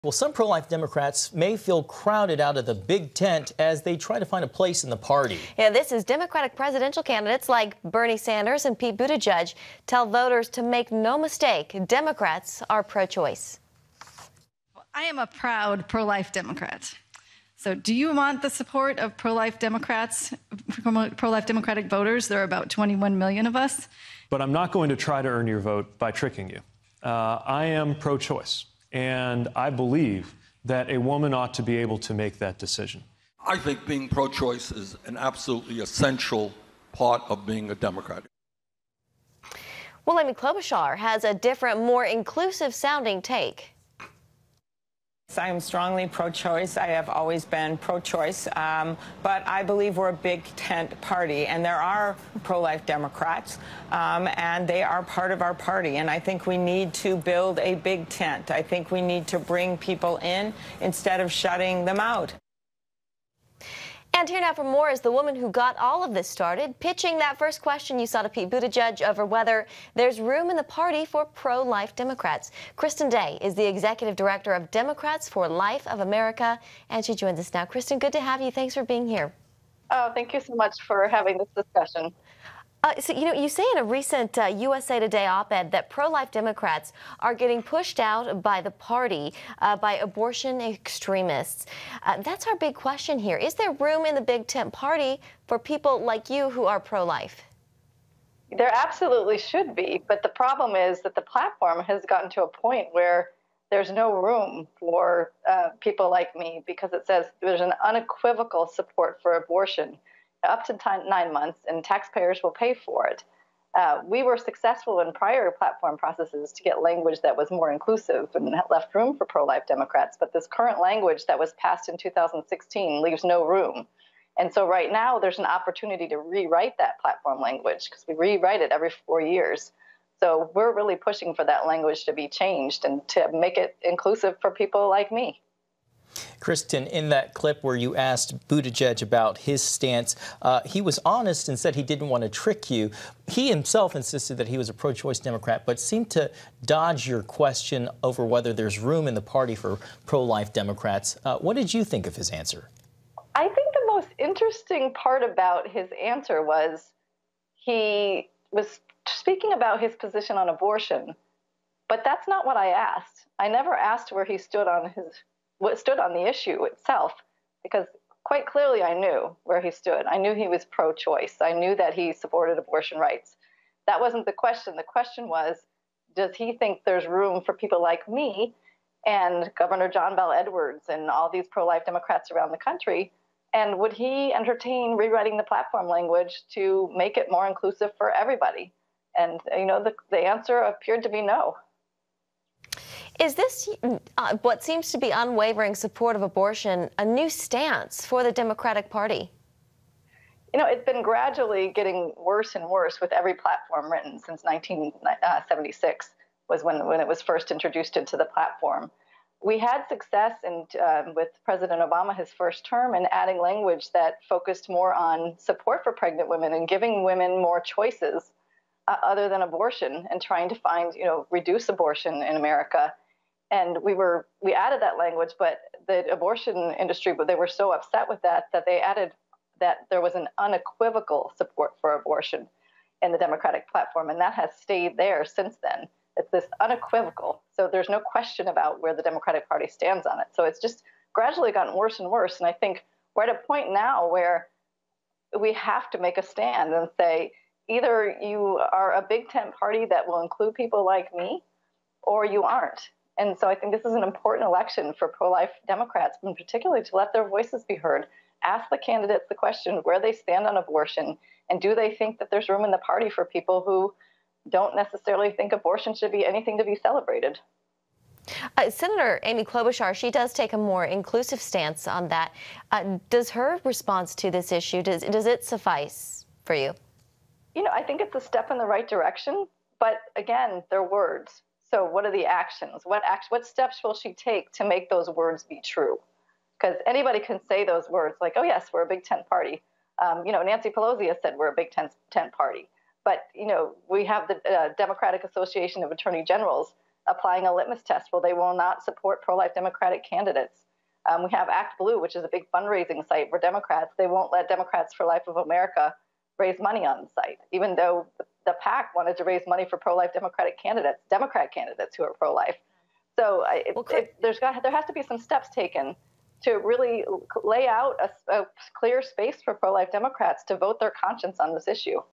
Is There Room for Us? Pro-Life Democrats Keep Asking Their Presidential Candidates, Only One Says '
Is There Room for Us? Pro-Life Democrats Keep Asking Their Presidential Candidates, Only One Says 'Yes'

What's coming up next?